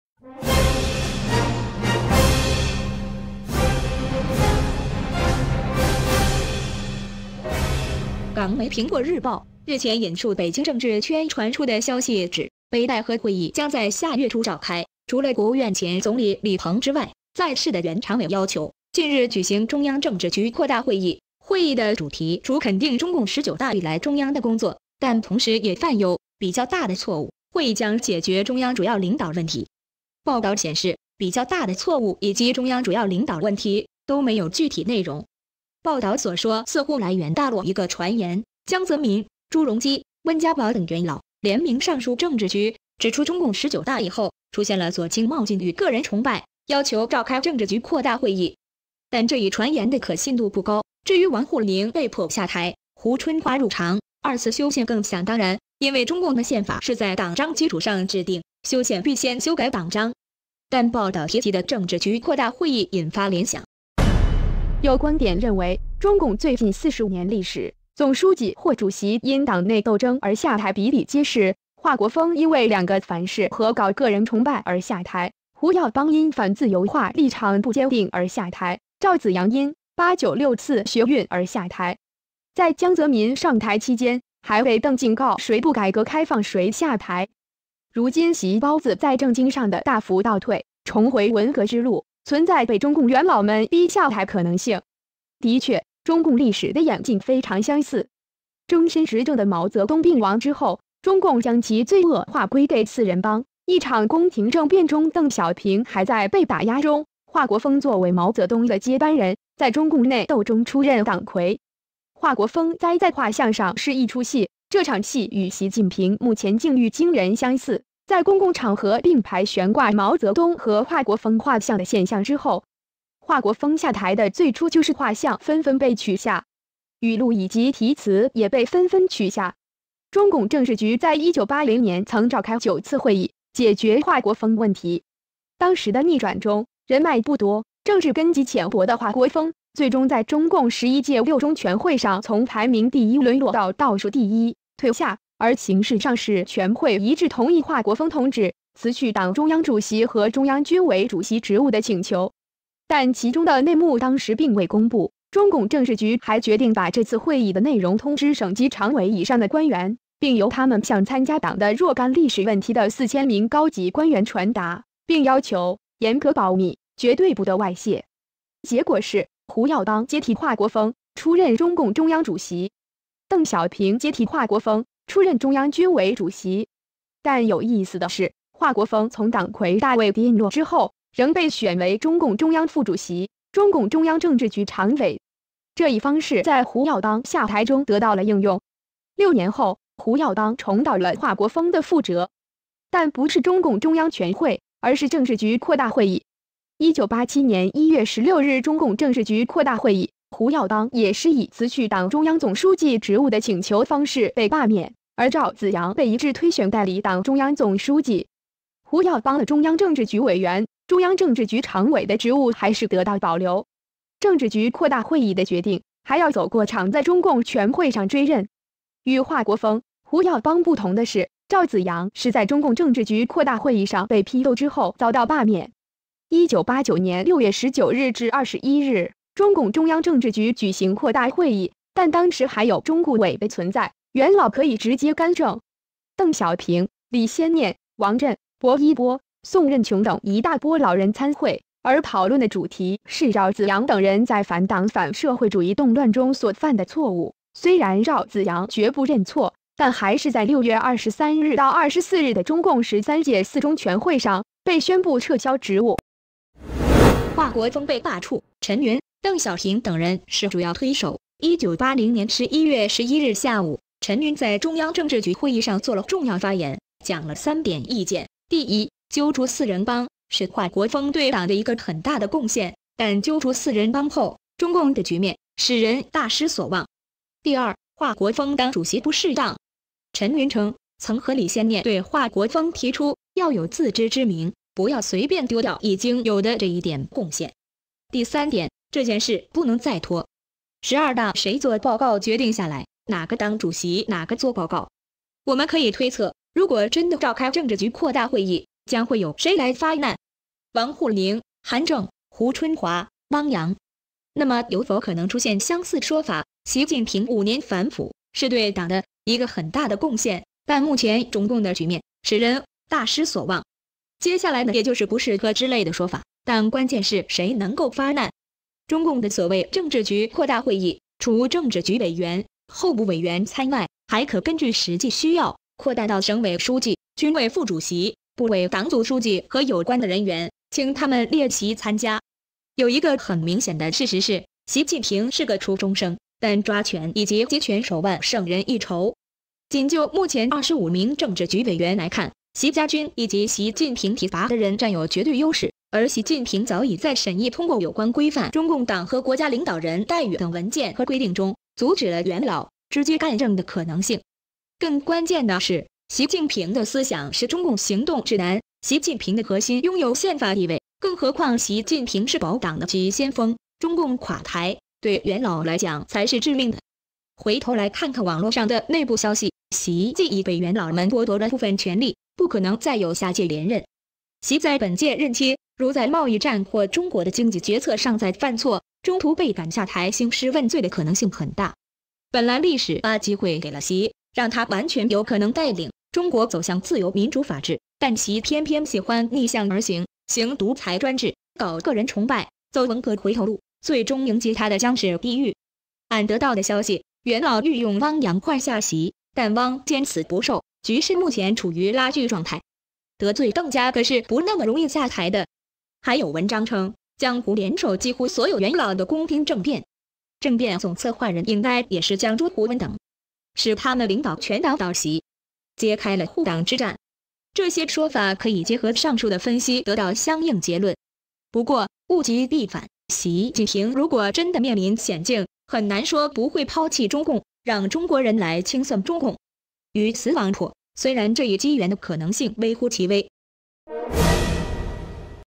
北戴河会议前有政治局扩大会议？习近平重蹈华国锋覆辙？
港媒苹果日日前引述北京政治圈传出的消息指，北戴河会议将在下月初召开，除了国务院前总理李鹏之外，在世的原常委要求，近日举行中央政治局扩大会议，会议的主题除肯定中共十九大以来中央的工作，但同时也犯有「比较大的错误」，会议将解决中央主要领导问题。

报导显示，比较大的错误以及中央主要领导问题都没有具体内容。

报导所说似乎来源大陆一个传言，江泽民、朱镕基、温家宝等元老，联名上书政治局，指出中共十九大以后出现了左倾冒进与个人崇拜，要求召开政治局扩大会议。